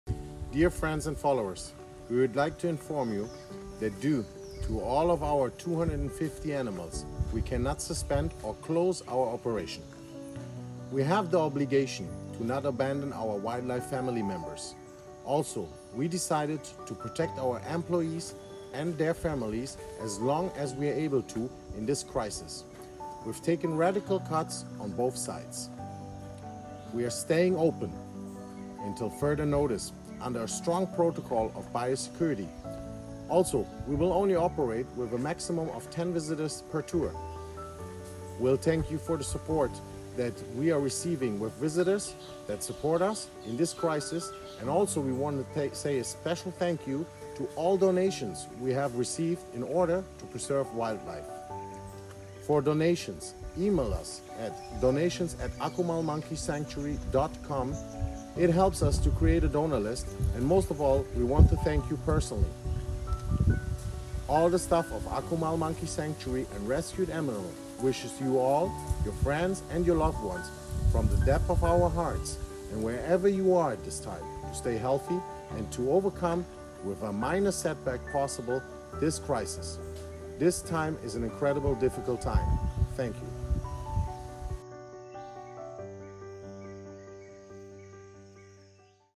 NOW IS THE TIME WE NEED YOUR HELP!
Akumal Monkey Sanctuary & Rescued Animals
Dear Friends and Followers we would like to inform you that, due to all of our over 250 Animals we can not Suspend or close our Operation. We have the obligation to not abandon our Wildlife Family members. Also, we decided to Protect our Employees and their Families as long as we are able to in this Crisis by taking radical cuts on both sides.

We are staying open until further notice under a strong Protocol of Biosecurity.
Also, we will only opérate with a Maximum of 10 visitors per Tour.

We thank you for the support that we are receiving with visitors that support us in this crisis and also we wanna say a special THANKS to all the Donation we have received in order to Preserve Wildlife.

It helps us create a donor list and most of all, we want to thank you personally!

All the Staff of Akumal Monkey Sanctuary & Rescued Animals, wish you all, your friends and loved ones, from the depths of our hearts and wherever you are at this time, to stay healthy and to overcome with the minor setbacks possible this crisis.
This time is an incredibly difficult time.